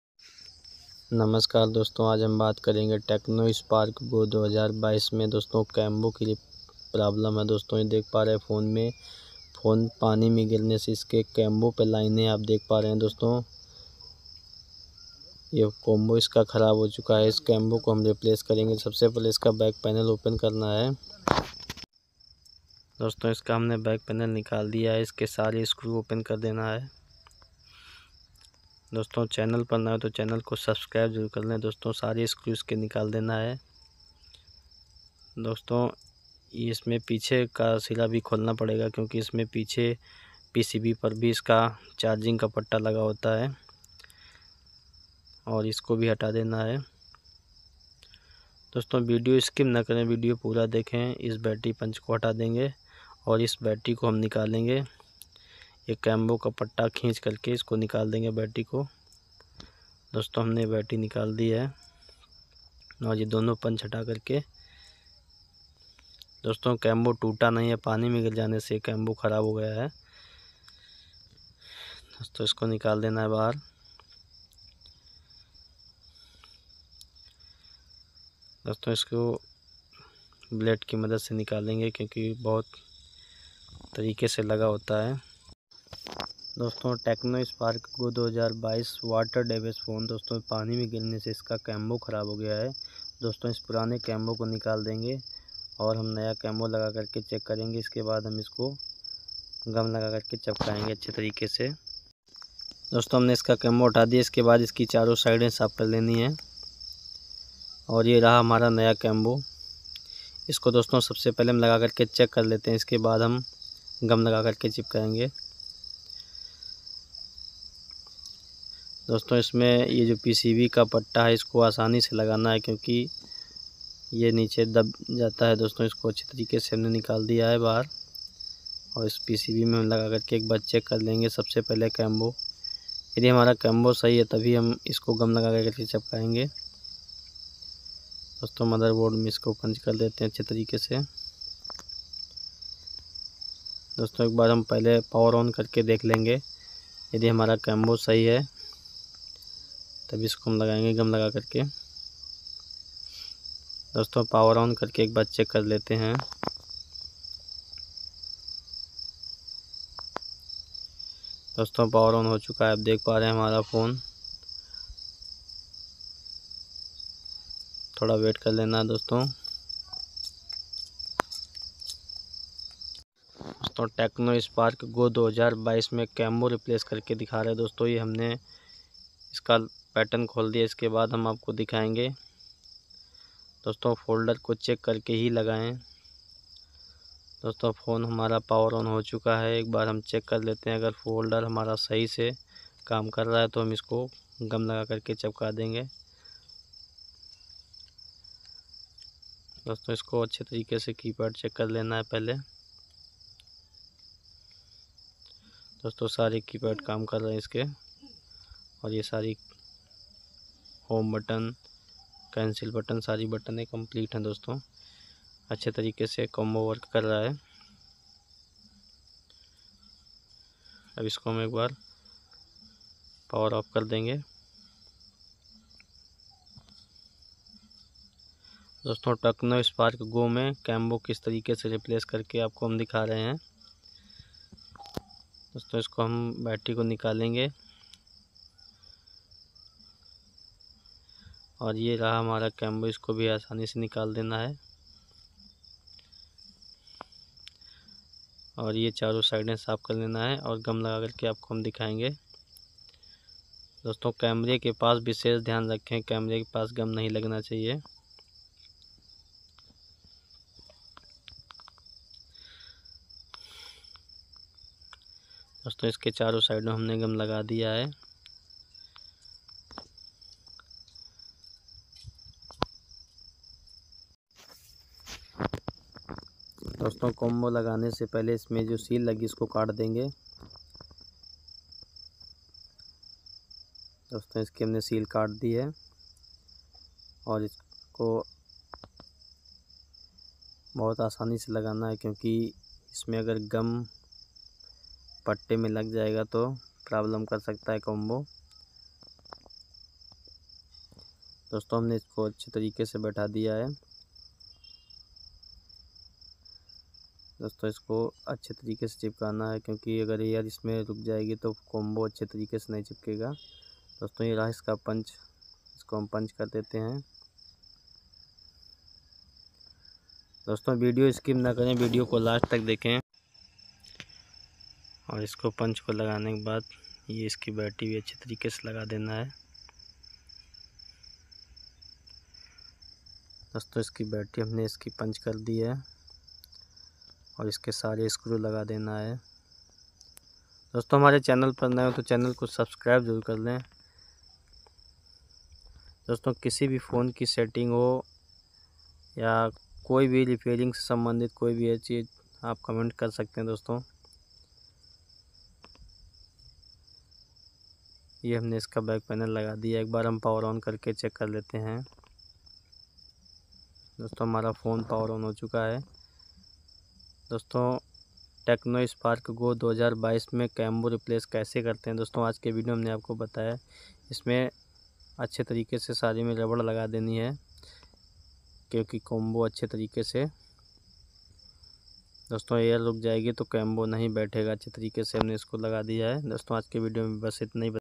Tecno Spark Go 2022 Screen Replacement | How to Open Tecno Spark Go 2022 Folder Display Change
Tecno Spark go 2022 Dissembly and screen Combo change | how to open tecno Spark ko 2022 Screen Replacement |

Hello doston
aaj ke is video mein tecno Go 2022 mobile mein screen replacement kaise karte hain is video mein step bye step dikhaya hai |

please is video ko jarur jarur dekhen  |